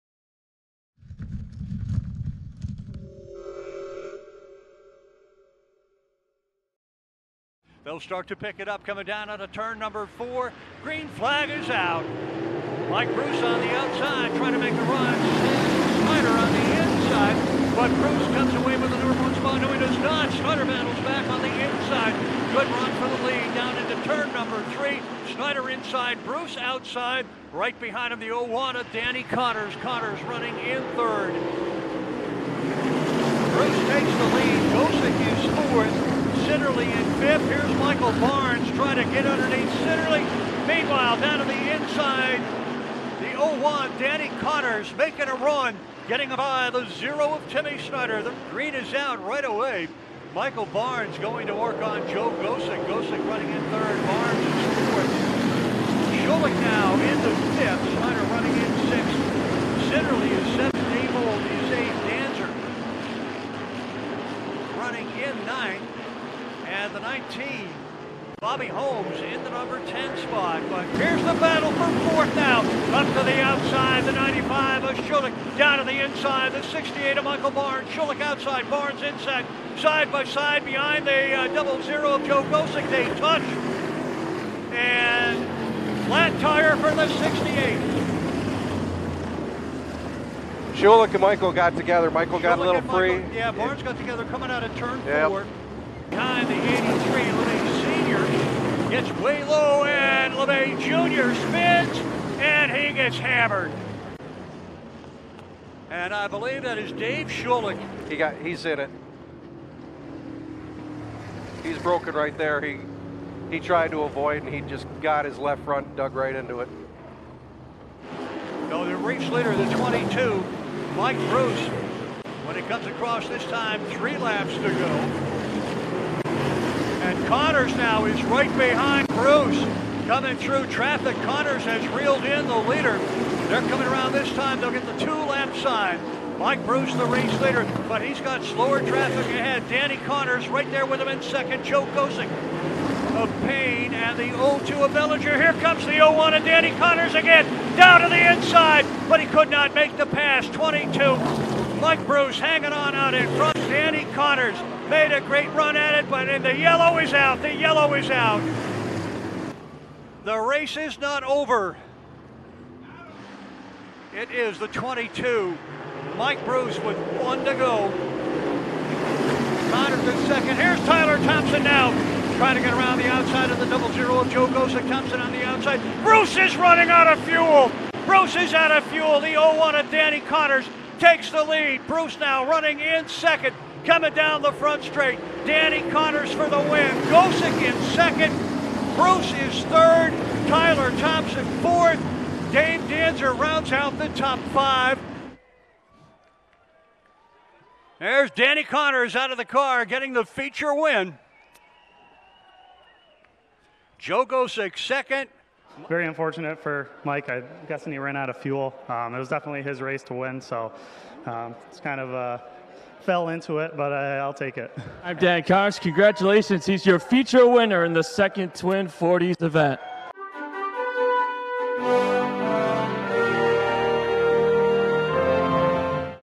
Novelis Supermodified Twin 40 #2 Highlights - August 3, 2024
Highlights from the second 40-lap Novelis Supermodified feature held on August 3, 2024 at Oswego Speedway.